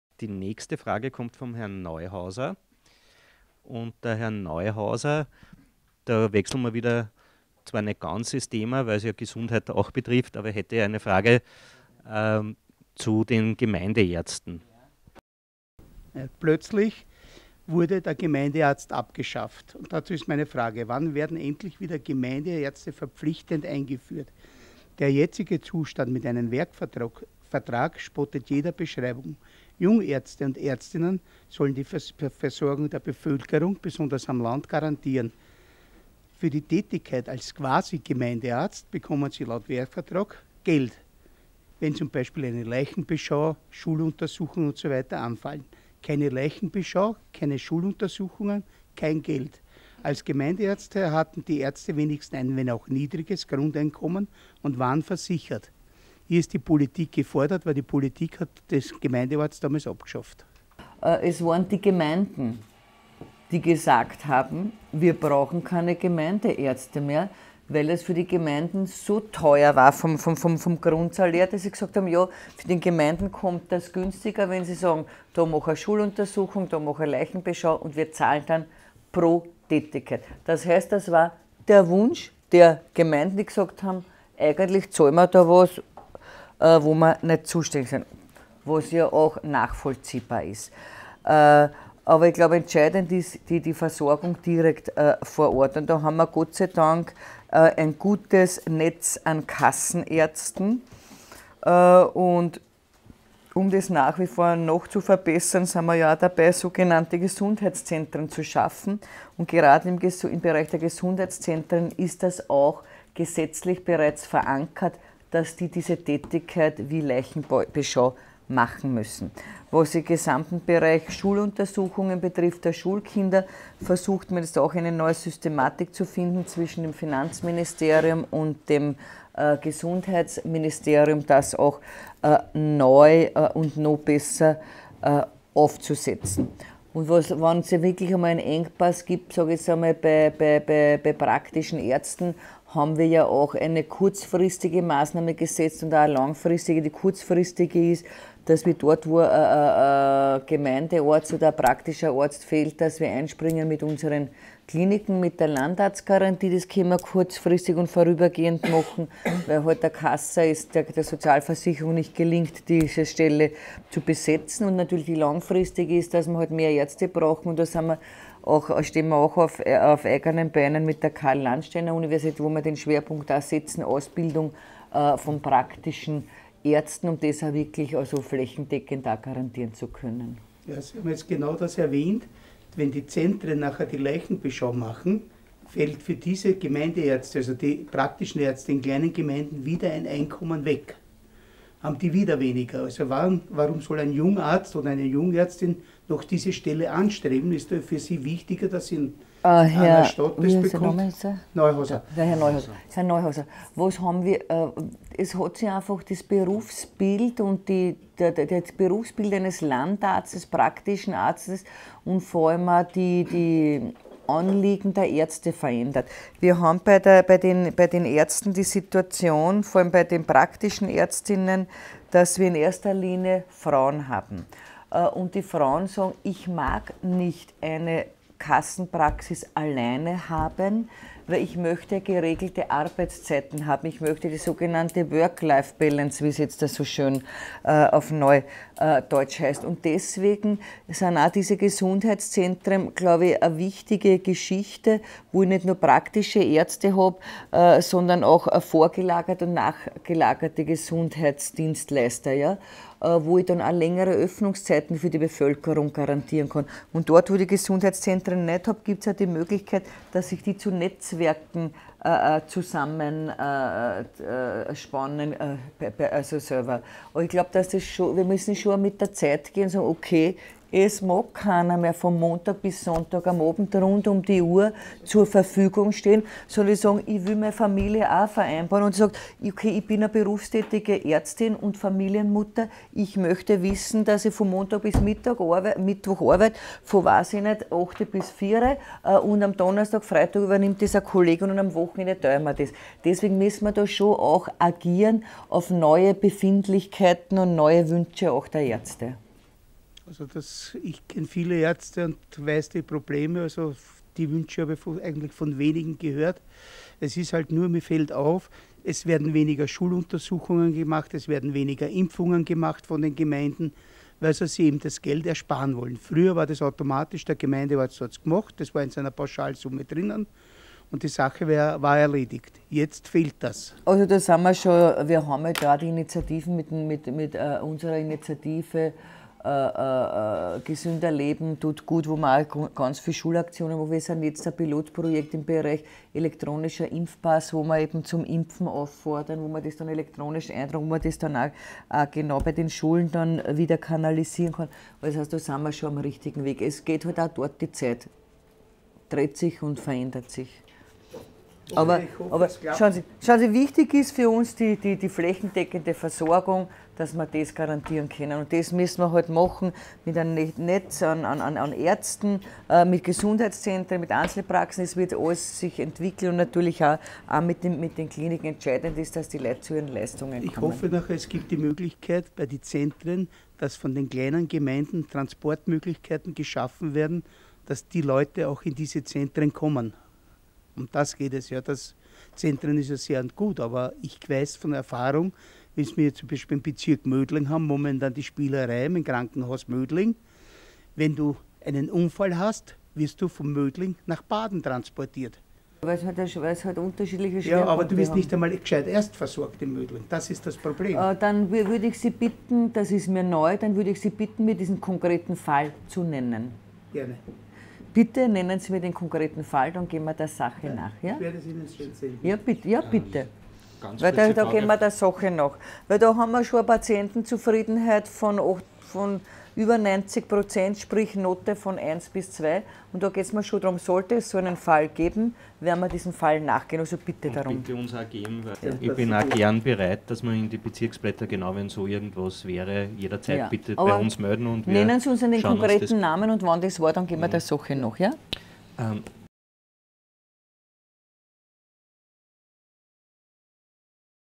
Gipfelgespräch Nebelsteinhütte mit Johanna Mikl-Leitner – Frage 5
Landeshauptfrau Johanna Mikl-Leitner beantwortete die Fragen der Bezirksblätter-Leser auf der Nebelsteinhütte.